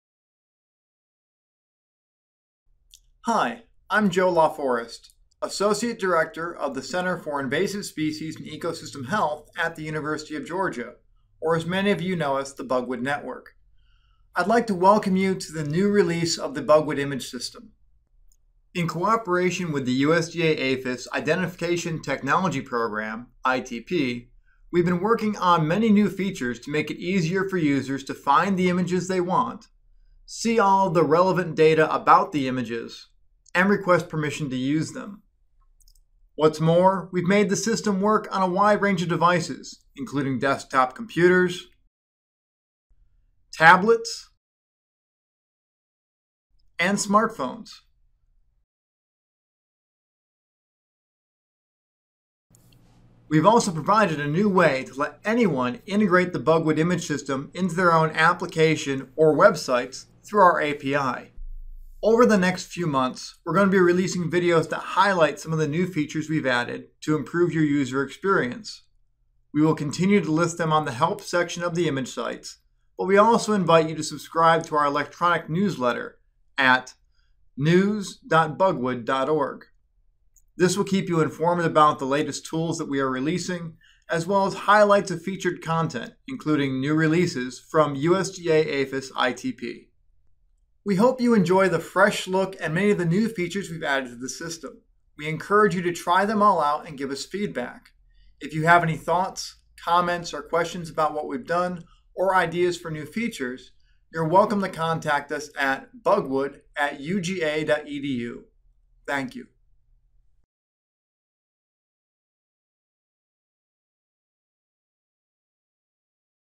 The New Bugwood Image System
In cooperation with USDA APHIS Identification Technology Program, we are proud to release a new user interface for the Bugwood Image Database.  This enhancement includes many new features designed to improve the experience of our users and make it easier to find images, see the information about them, and use them.